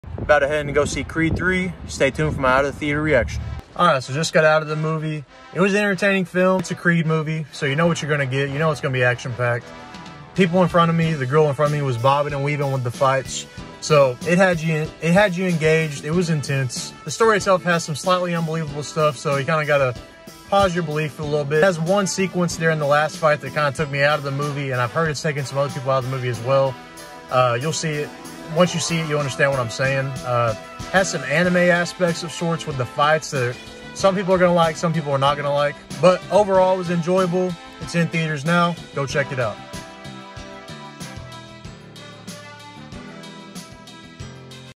Creed III out of theater reaction
Creed 3 Out of the theater reaction. Creed III stars Michael B Jordan and Johnathan Majors, and finishes the trilogy for Adonis Creed, created from the original Rocky Franchise starring Sylvester Stallone as Rocky Balboa.